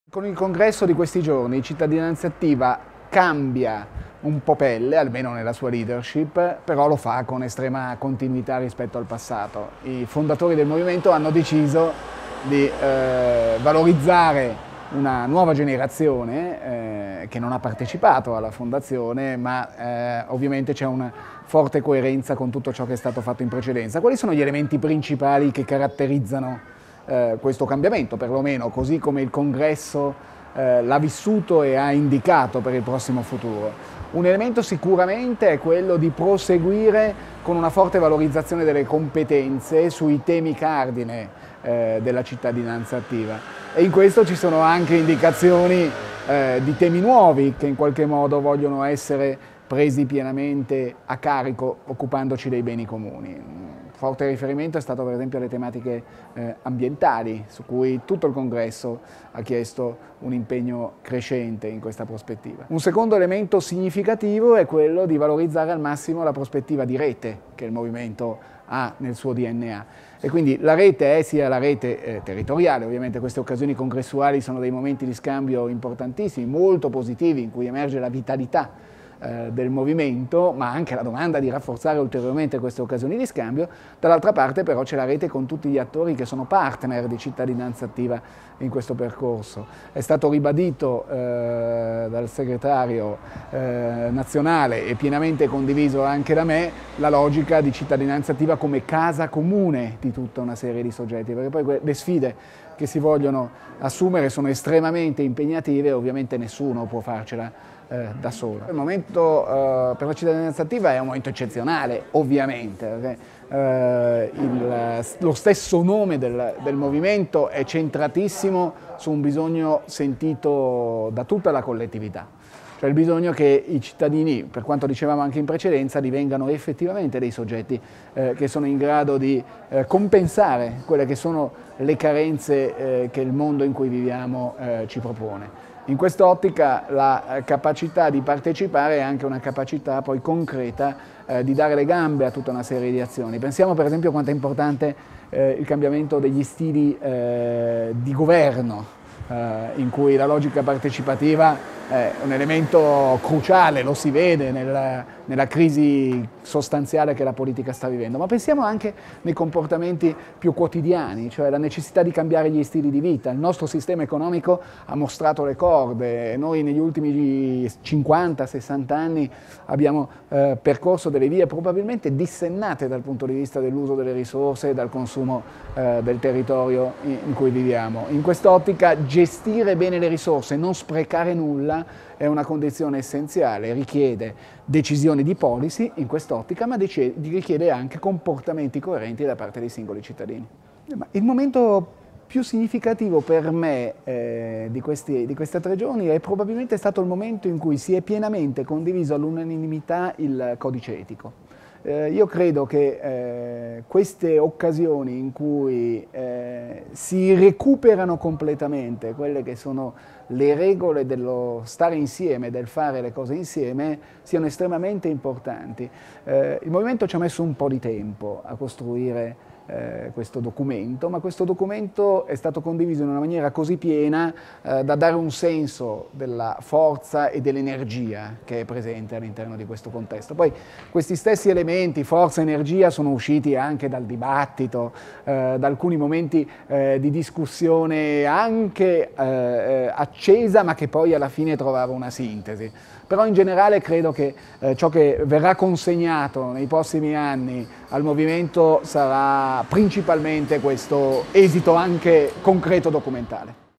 Intervista al neo Presidente nazionale di Cittadinanzattiva, Marco Frey
Intervista a Marco Frey, eletto Presidente Nazionale di Cittadinanzattiva al termine del IV Congresso nazionale "Italia:punto e a capo", tenutosi a Chianciano Terme dal 21 al 24 giugno 2012.
Marco Frey, laureato nel 1986 in Discipline Economiche e Sociali all'Università Bocconi, professore ordinario presso la Scuola Superiore S. Anna di Pisa, subentra ad Alessio Terzi. Frey è da tempo impegnato nel mondo universitario, in quello civico e a supporto delle Istituzioni sui temi della tutela e della salvaguardia dell'ambiente, nella tutela dei diritti umani e nella lotta alla corruzione, sulle questioni inerenti la sicurezza e salute sul lavoro.